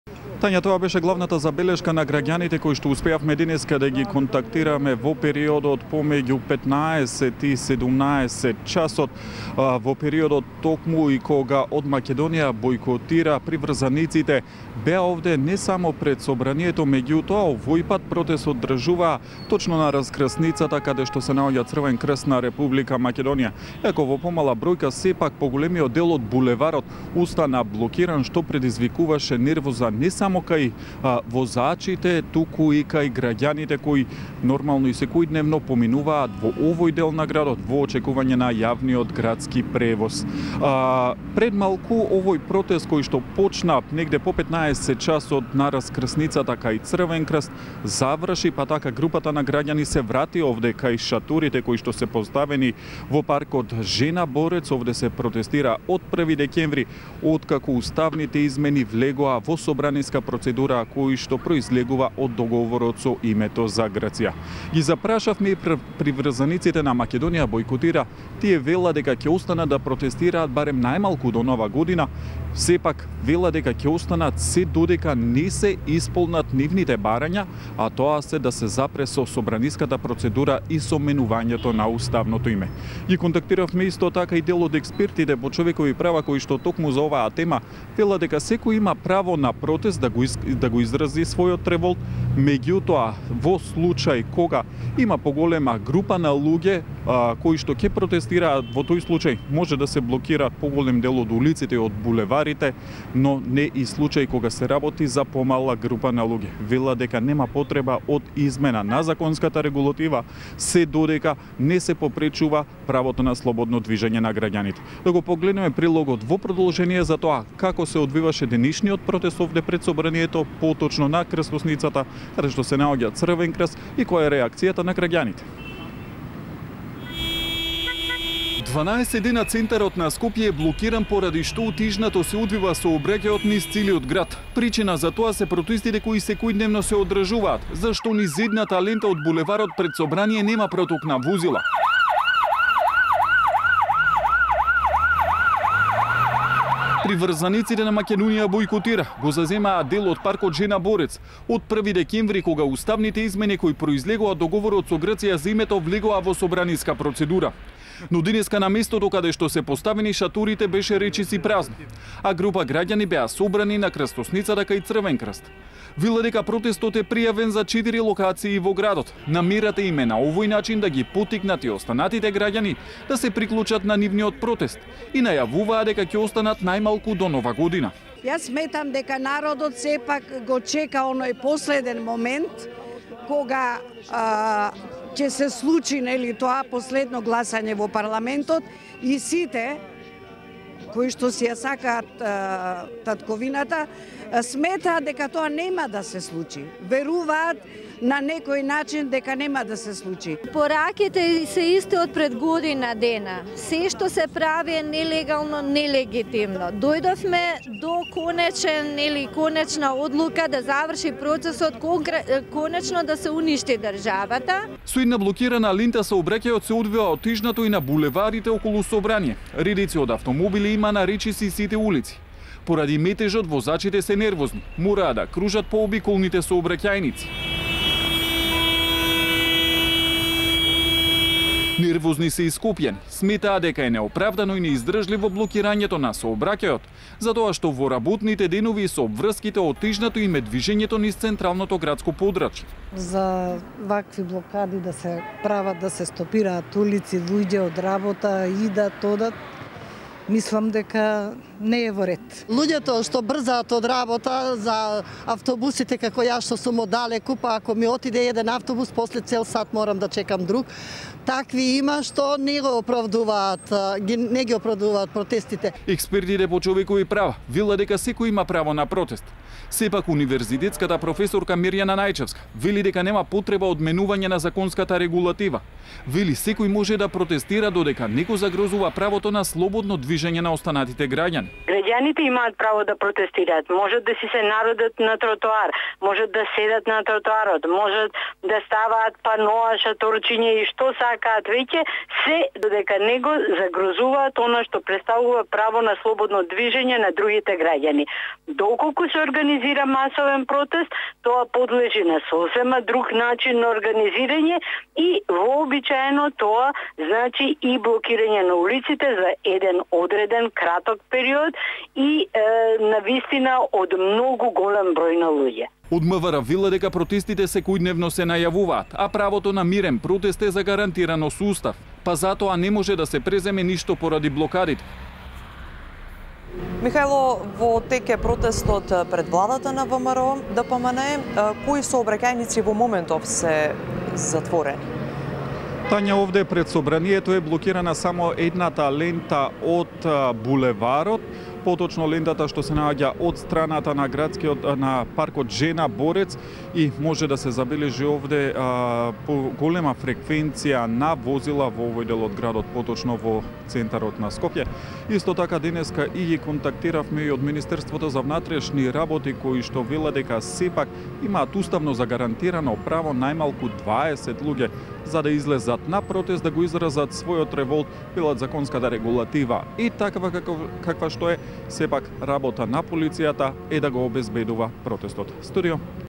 Скопје со денови е парализирано до неколку десетици граѓани
Во центарот на Скопје и денеска беа блокирани некои од клучните крстосници во шпицот на денот, што предвикува голема нервоза кај граѓаните.. Синоќа, за малку беше избегнат посериозен инцидент.. Михајло Видмлиски е пред Собранието.